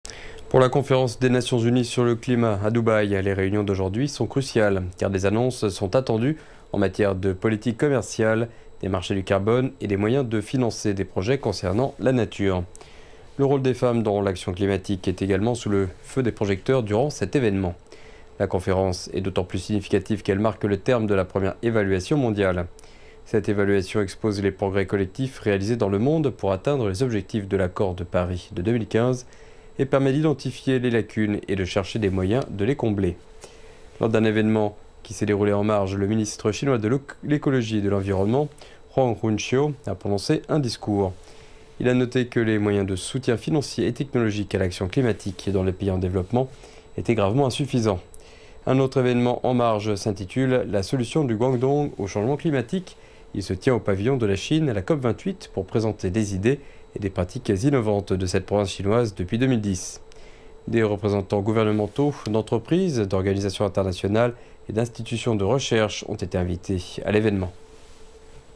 Le financement de la question climatique au centre des débats de la COP28
Pour la Conférence des Nations Unies sur le climat à Dubaï, les réunions de lundi sont cruciales, car des annonces sont attendues en matière de politique commerciale, de marchés du carbone et de moyens de financer des projets concernant la nature. Le rôle des femmes dans l'action climatique est également sous les projecteurs durant cet événement. La conférence est d'autant plus significative qu'elle marque le terme de la première évaluation mondiale.